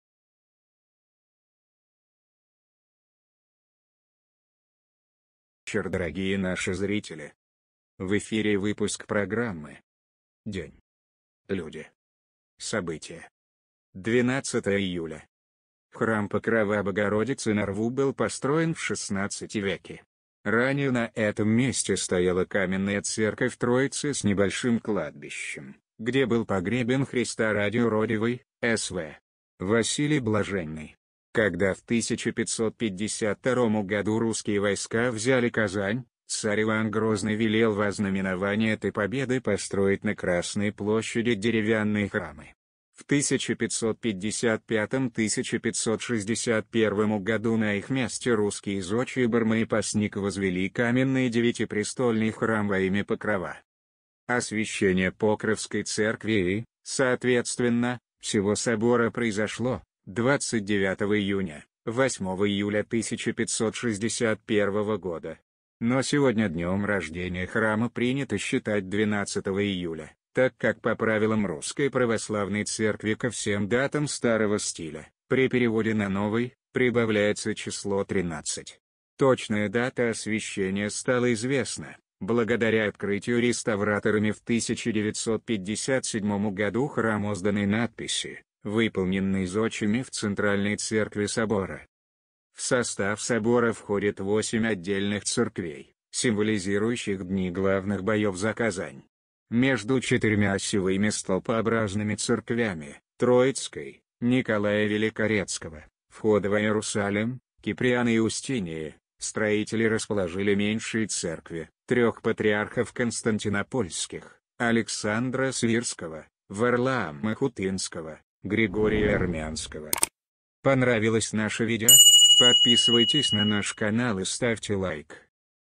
День Люди События. 12 июля.На Красной Площади в Москве освящен Покровский собор
На Красной Площади в Москве освящен Покровский собор, известный также как Храм Василия Блаженного
Храм Покрова Богородицы на Рву был построен в 16 веке. Ранее на этом месте стояла каменная церковь Троицы с небольшим кладбищем, где был погребен Христа Ради юродивый, Св. Василий Блаженный. Когда в 1552 году русские войска взяли Казань, царь Иван Грозный велел в ознаменование этой победы построить на Красной площади деревянные храмы. В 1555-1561 году на их месте русские зодчие Барма и Постник возвели каменный девятипрестольный храм во имя Покрова.
Освящение Покровской церкви и, соответственно, всего собора произошло (29 июня) 8 июля 1561 года. Но сегодня «днем рождения» храма принято считать 12 июля (так как по правилам Русской Православной церкви ко всем датам старого стиля, при переводе на новый, прибавляется число 13). Точная дата освящения стала известна, благодаря открытию реставраторами в 1957 году храмозданной надписи, выполненной зодчими в центральной церкви собора.
В состав собора входят восемь отдельных церквей, символизирующих дни главных боев за Казань. Между четырьмя осевыми столпообразными церквями — Троицкой, Николая Великорецкого, Входа во Иерусалим, Киприана и Устинии — строители расположили меньшие церкви: Трех патриархов Константинопольских, Александра Свирского, Варлаама Хутынского, Григория Армянского.
Электронная книга «Раскрутка на YouTube»

ДеньЛюдиСобытия 12июля Красной Площади#Покровский собор#ЕкатеринаII#ТВИНФО